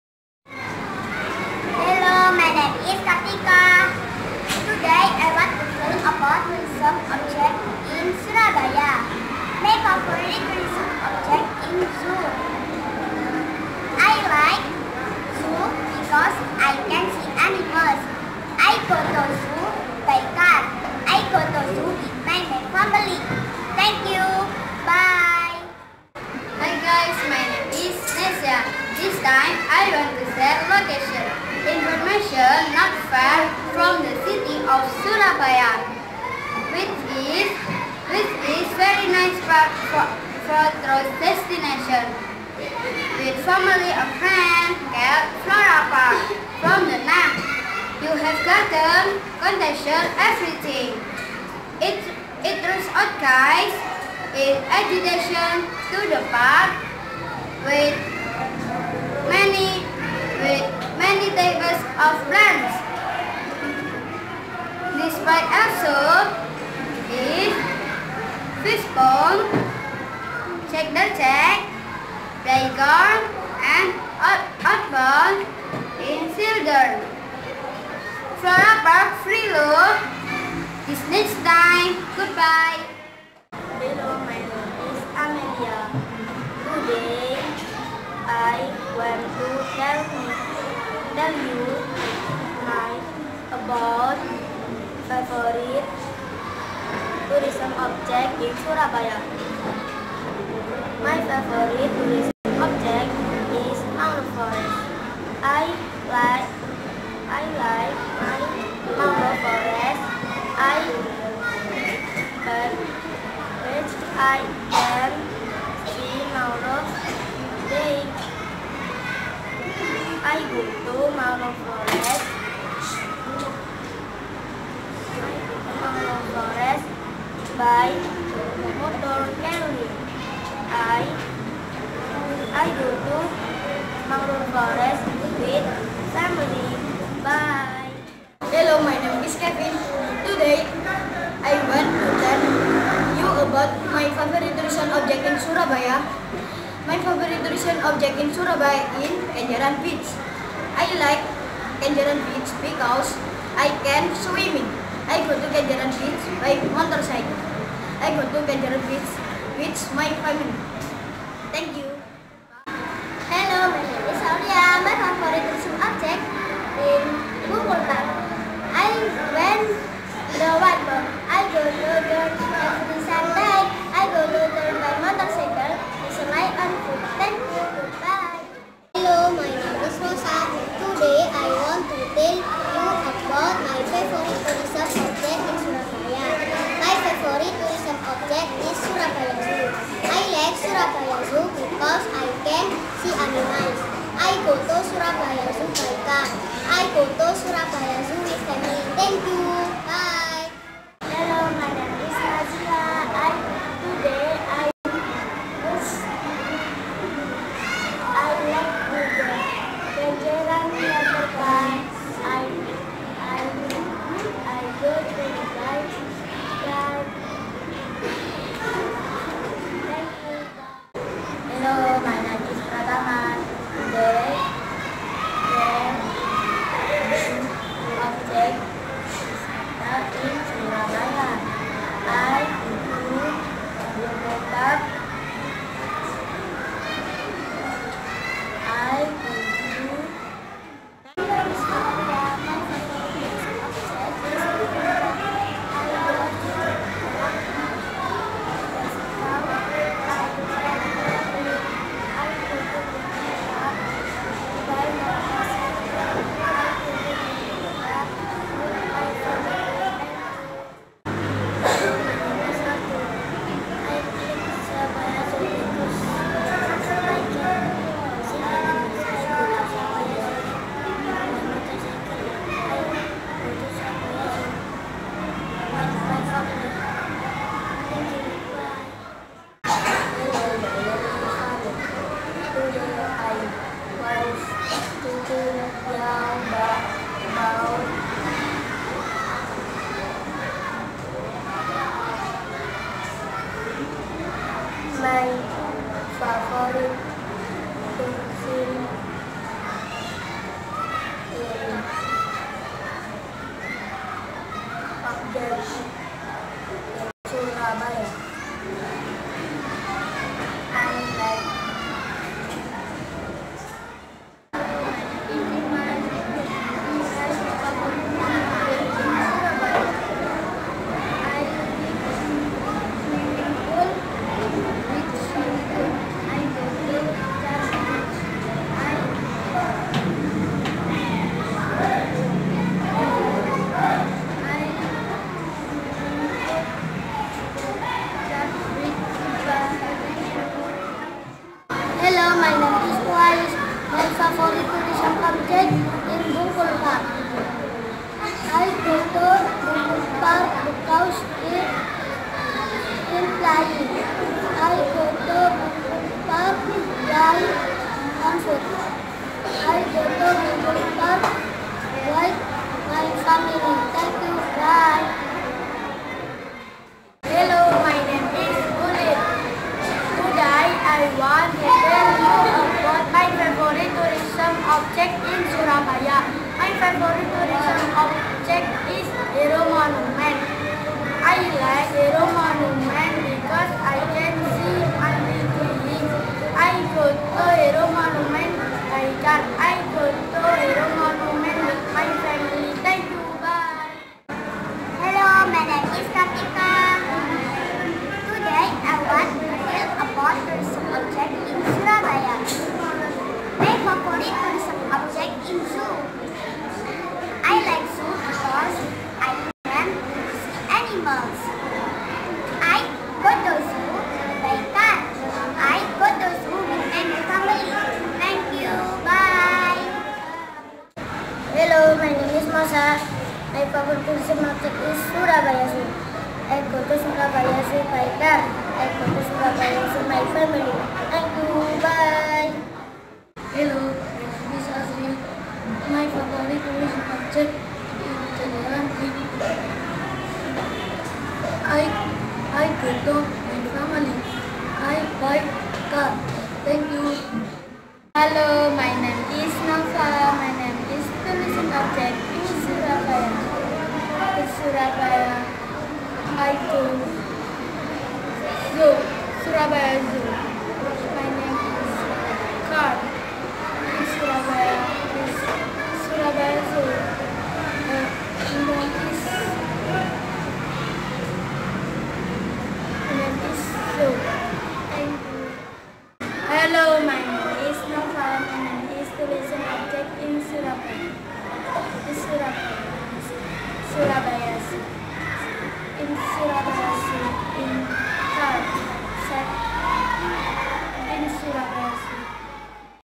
SDN NGAGEL I / 394 Surabaya | Telling About Yourism Object | Diorama
Menceritakan tentang objek wisata yang ada di Surabaya dengan menunjukkan media Diorama yang sudah dibuat dirumah